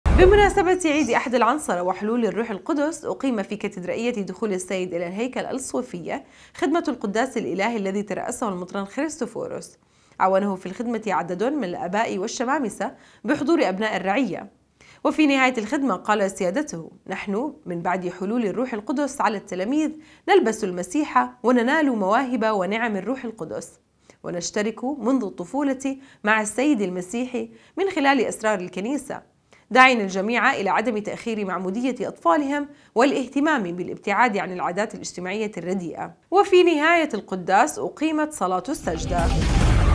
القدّاسِ الإلهيّ بمناسبةِ عيدِ أحدِ العنصره وحلولِ الروح القدس في كاتدرائيةِ دخولِ السيد الى الهيكل
بمناسبةِ عيدِ أحدِ العنصره وحلولِ الروح القدس أقيم في كاتدرائيةِ دخولِ السيد الى الهيكل الصويفيه خدمةُ القدّاسِ الإلهيّ الذي ترأسَه المطران خريستوفوروس / عاونَهُ في الخدمةِ عددٌ من الآباءِ والشمامسه بحضورِ أبناءِ الرعيه / وفي نهايةِ الخدمةِ قال سيادَتُه نحن من بعدِ حلولِ الرّوحِ القدسِ على التلاميذ نلبسُ المسيحَ وننالُ مواهبَ ونِعَمَ الروحِ القدس / ونشتركُ منذُ الطّفولةِ مع السيدِ المسيحِ من خلالِ أسرارِ الكنيسه داعياً الجميعَ إلى عدمِ تأخيرِ معموديةِ أطفالِهم والاهتمامِ بالابتعادِ عن العاداتِ الاجتماعيةِ الرديئه/ وفي نهايةِ القدّاسِ أقيمت صلاةُ السّجده